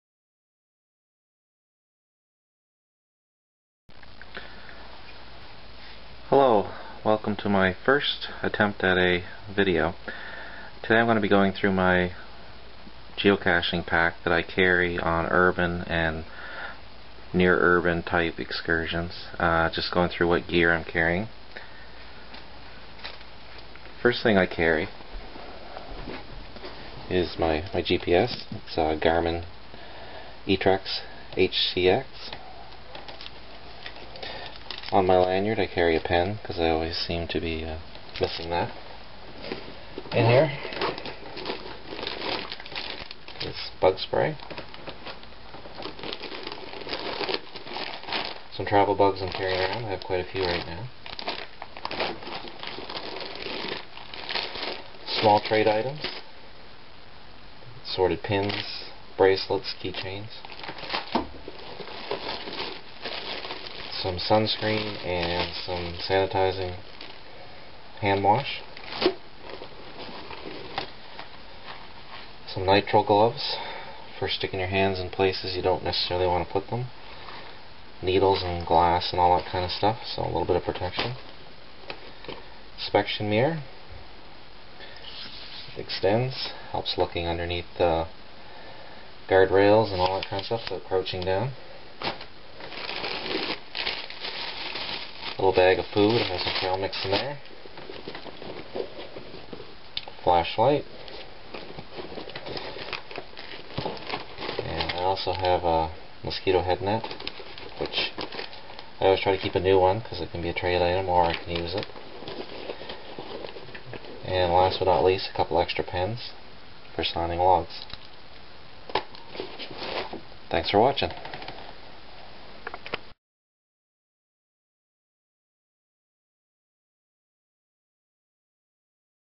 Urban Geo Pack
My pack and items to take while doing urban geocaching.